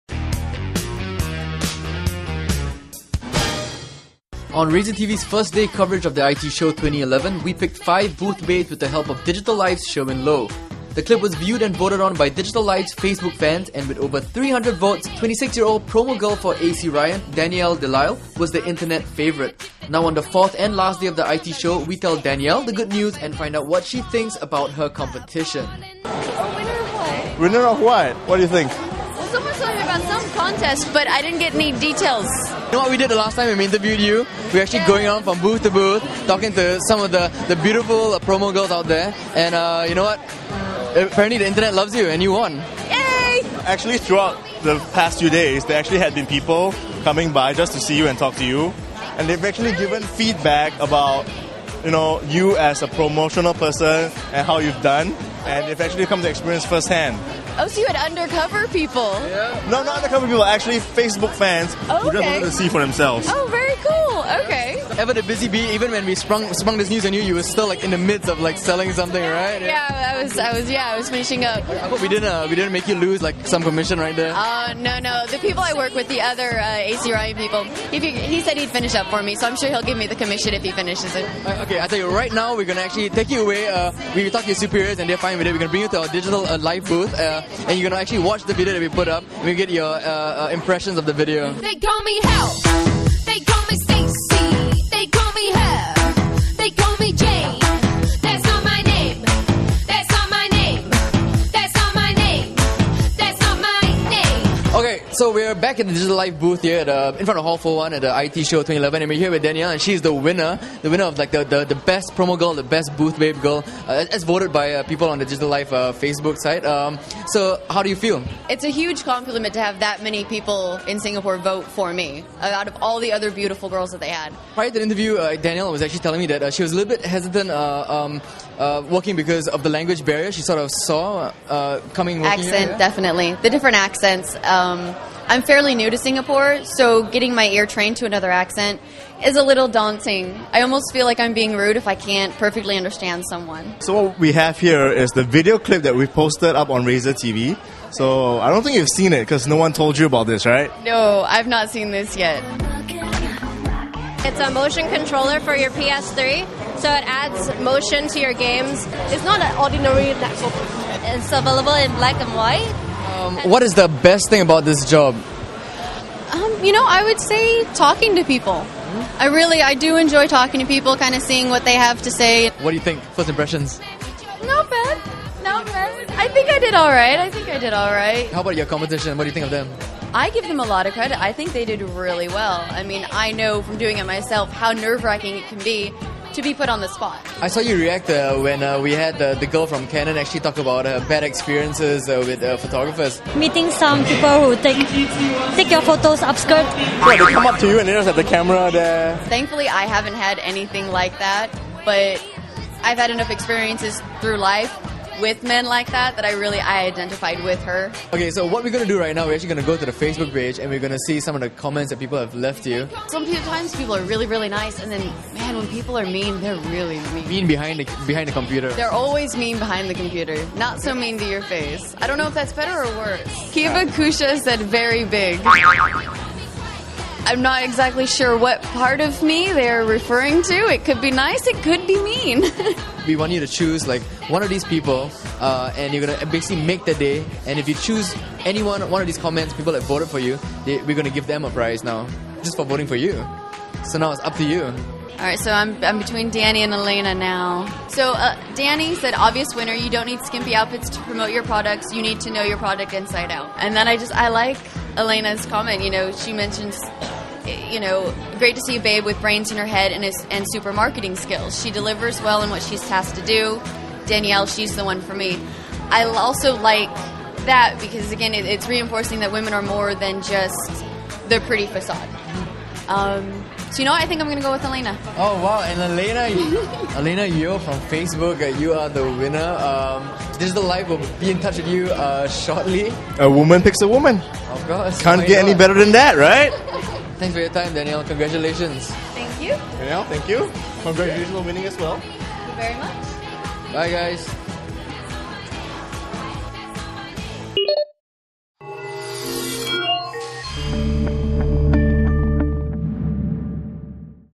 Most popular booth babe (IT Show 2011 Ep 4.2)
Digital Life and RazorTV picked five booth babes to showcase their skills. Digital Life fans voted and 26 year old Danielle DeLisle emerged the winner, we tell her the good news and get her to return the favour to one lucky Digital Life fan.